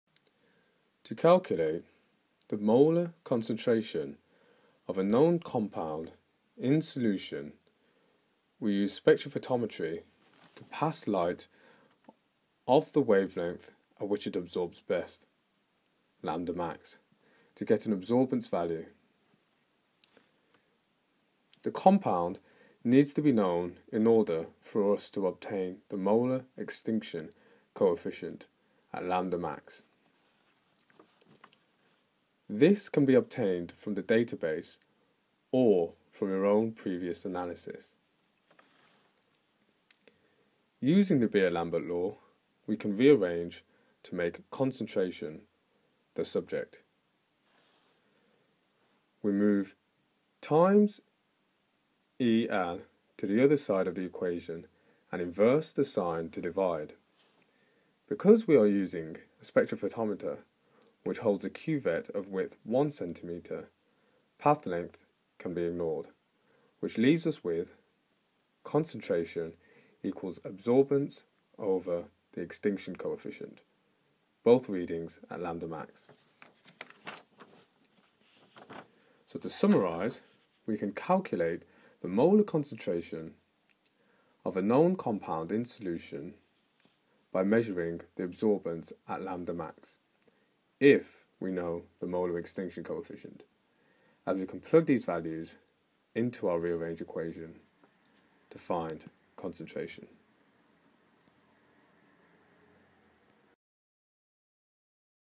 Beer Lambert Law - Concentration
Finding the concentration using the Beer Lambert law